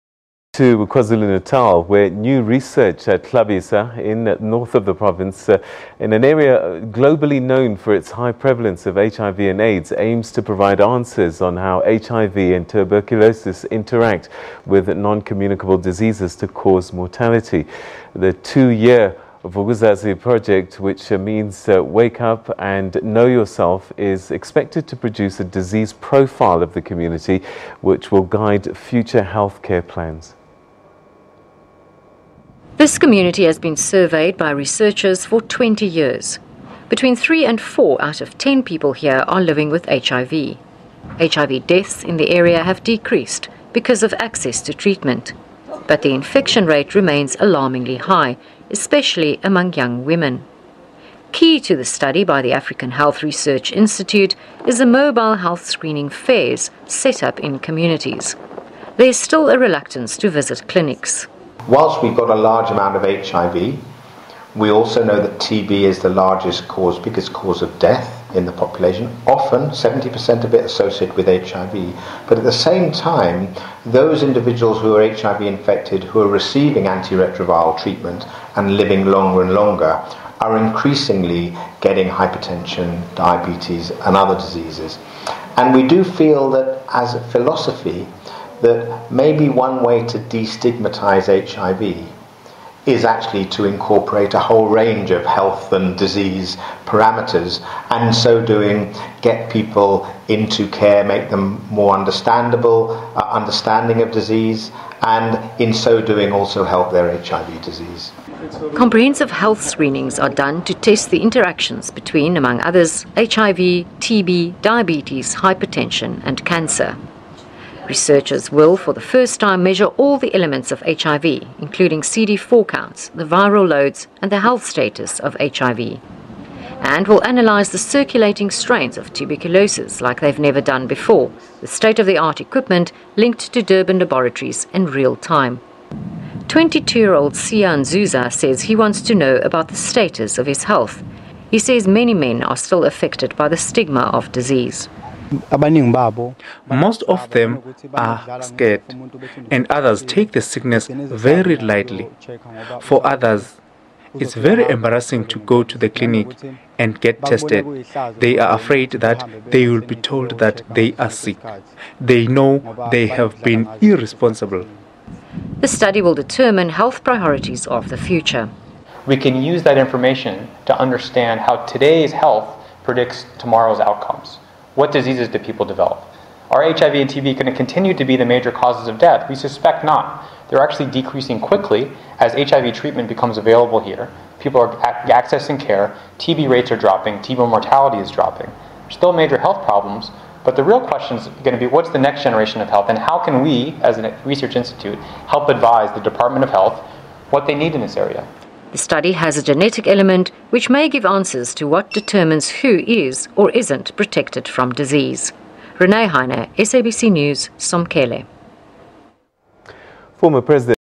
New research to shed light on HIV and TB in KZN
New research at Hlabisa in northern KwaZulu-Natal - an area globally known for its high prevalence of HIV/Aids - aims to provide answers on how HIV and Tuberculosis interact with non-communicable diseases to cause mortality. The two-year Vukuzazi project - which means wake up and know yourself - is expected to produce a disease profile of the community which will guide future health care plans.